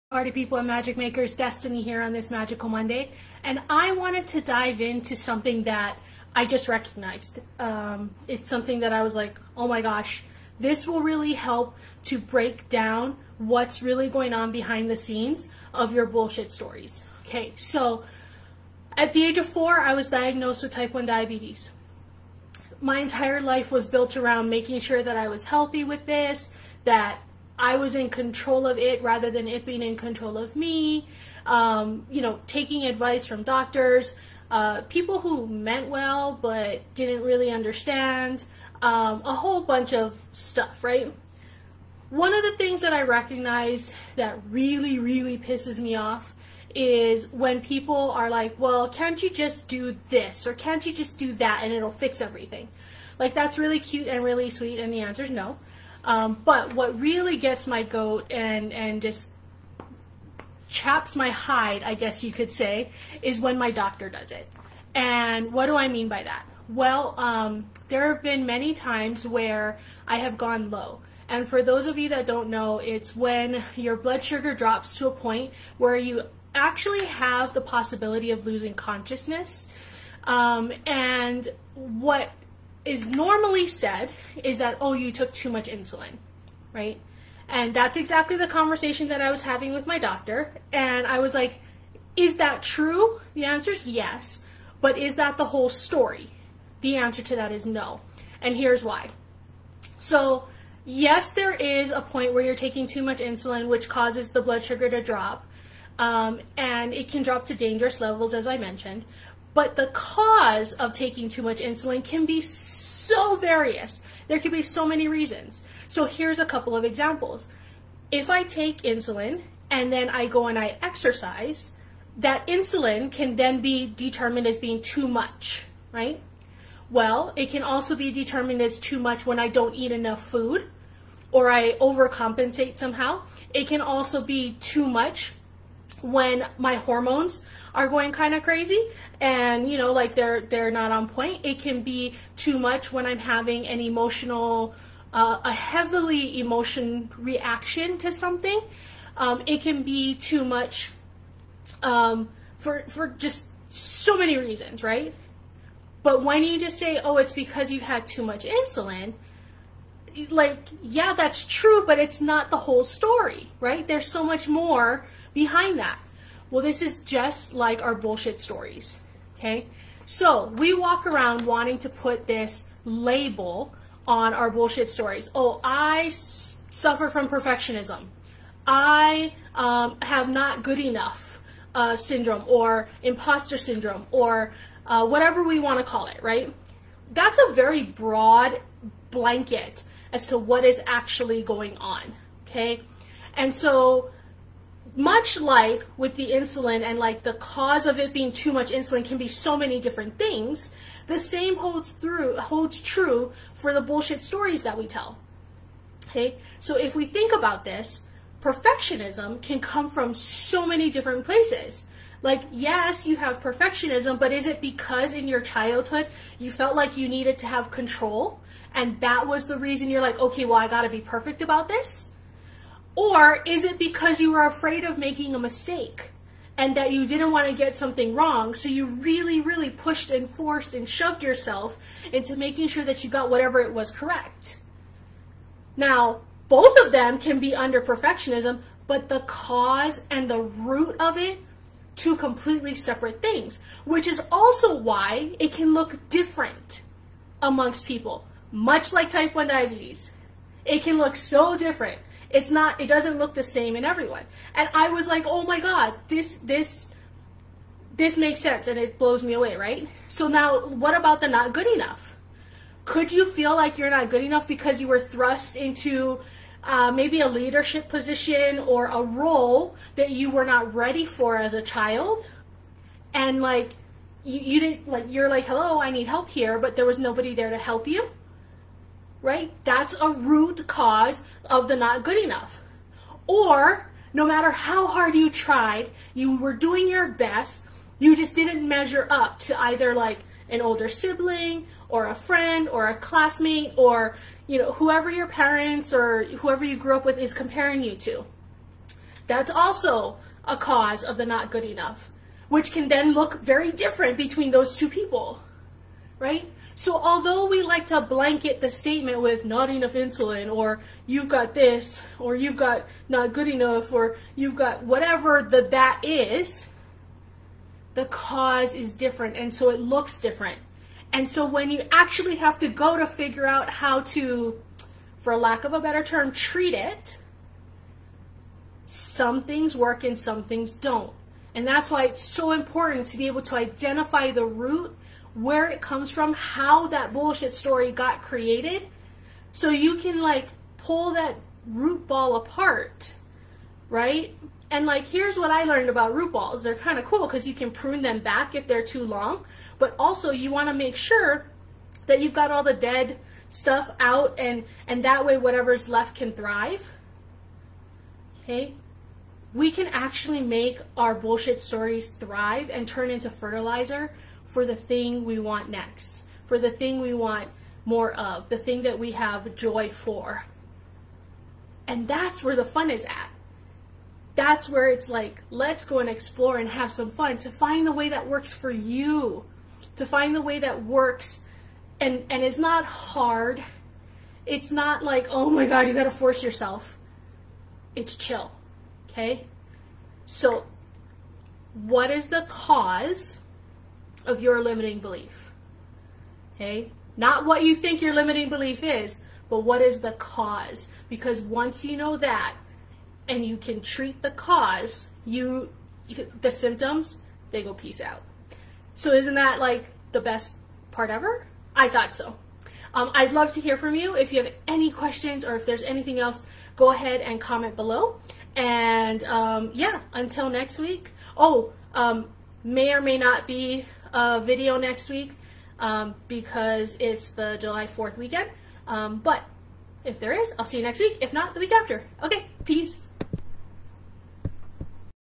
T1D and Your BS Stories
What does Type 1 diabetes and your bullshit stories have in common?
A lot more than you may think.
After living with T1D for 37 years I began to realize there are similarities with it and BS stories.
It's a wide net we cast and it doesn't serve us.
Check out this vid to learn more.